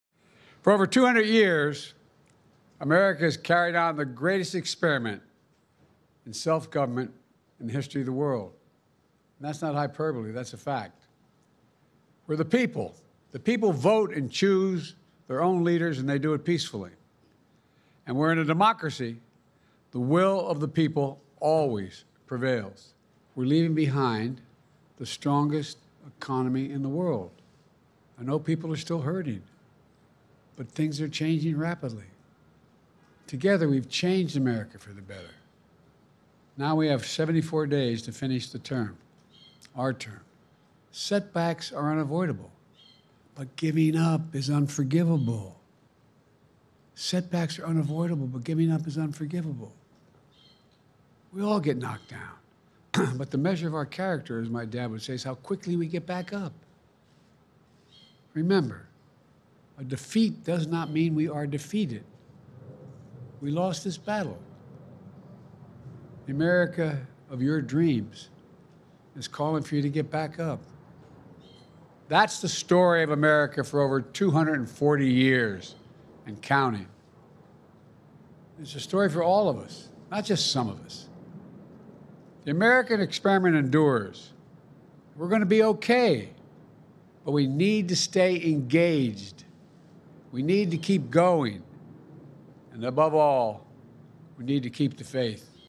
Joe Biden accepts defeat, full speech
In an address to the nation, Joe Biden said he accepted Donald Trump’s victory and vowed a peaceful transfer of power, as his press spokesman said he believed stepping down from the election was ‘the right thing for Americans’ .